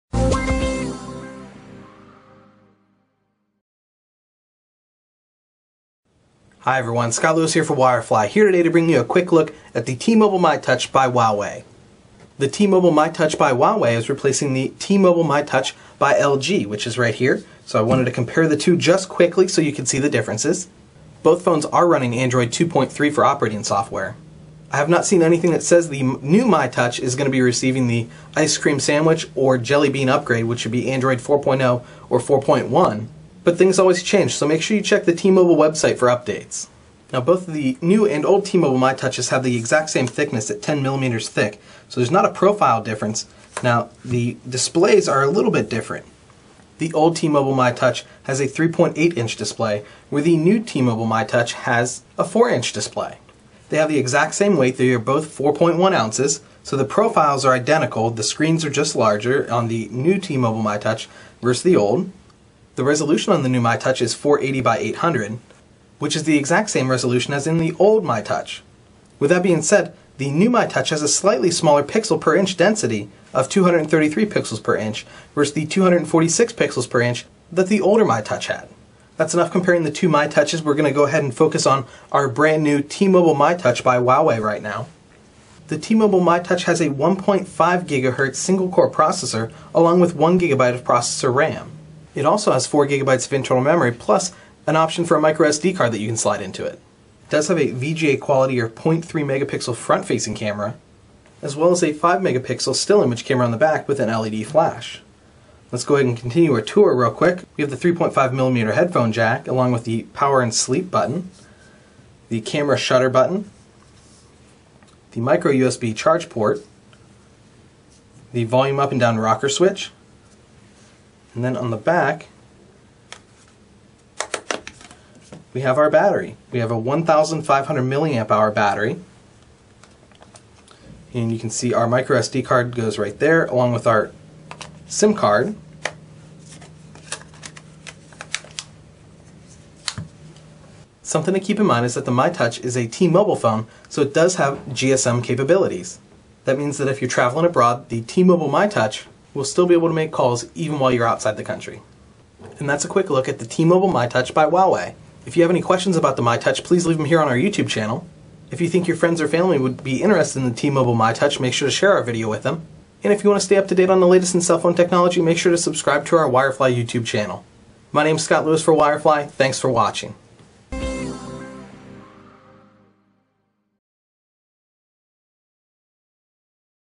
T-Mobile myTouch by Huawei Smartphone Quick Look Review from Wirefly
Take a quick look at the new T-Mobile myTouch smartphone by Huawei.

See how Huawei's new version of the myTouch stacks up against the previous T-Mobile myTouch by LG when Wirefly's Scott Lewis shows you some of the key differences between these two popular smartphones.